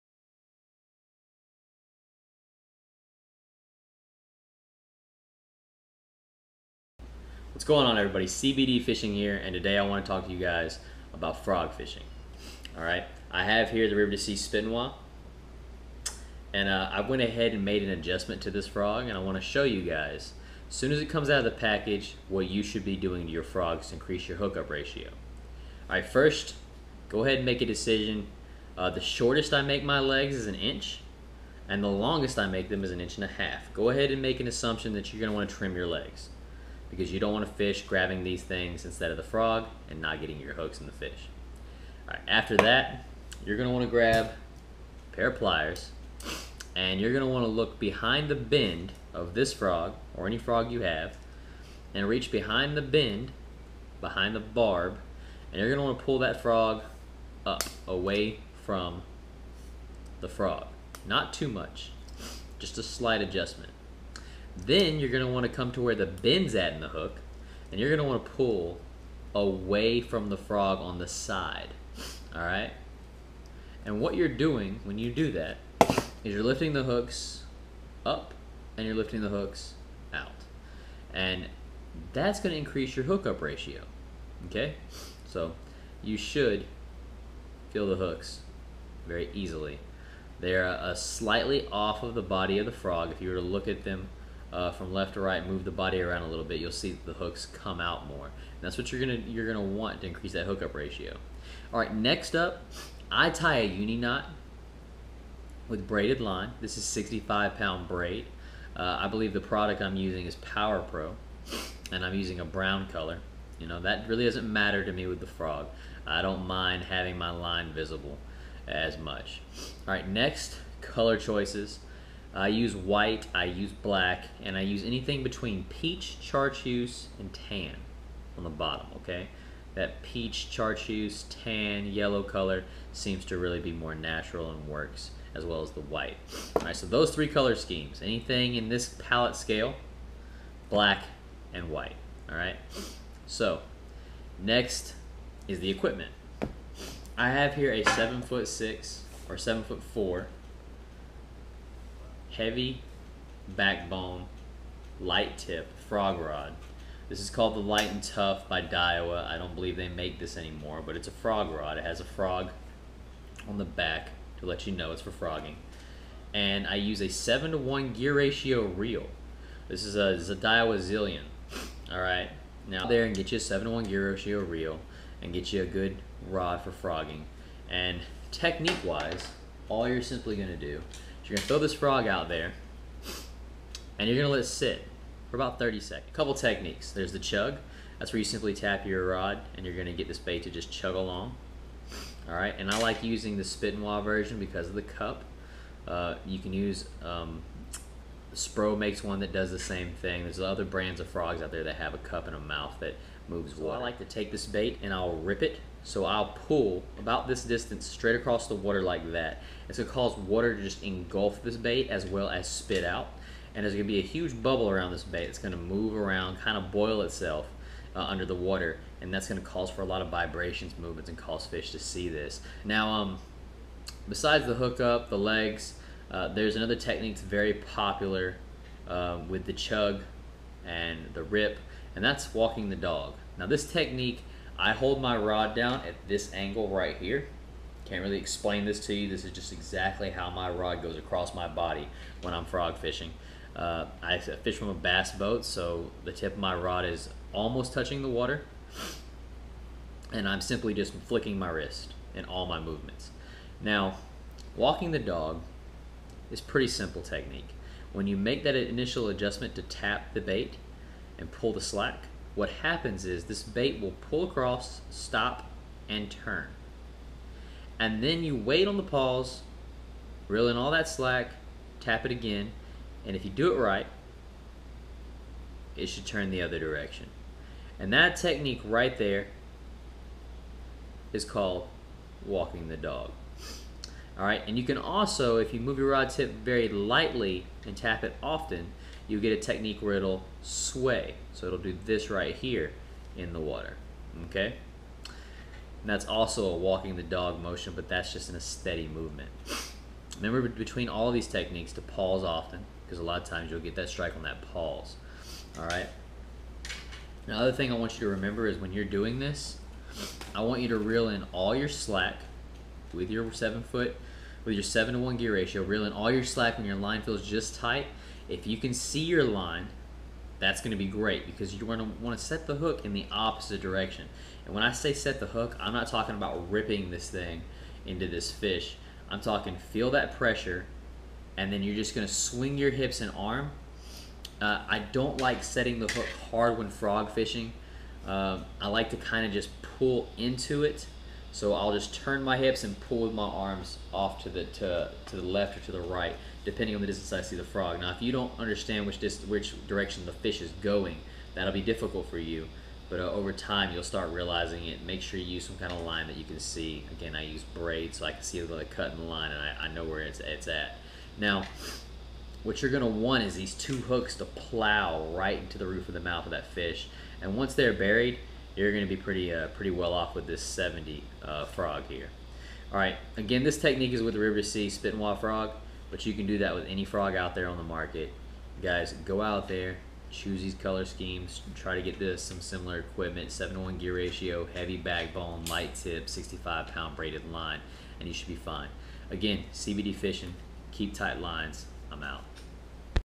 Tips and Techniques - How To Frog Fish
In this video, I discuss how to fish a hollow bodied top water frog. I show you what adjustments to make to ensure a higher percentage hook up ratio, as well as ways to fish the bait.

First we modify the skirted legs and the bend of the hook to increase the hook-up ratio. Next thing would be color selection and fishing equipment. Followed by the actual techniques used to attract bass with lifelike presentations.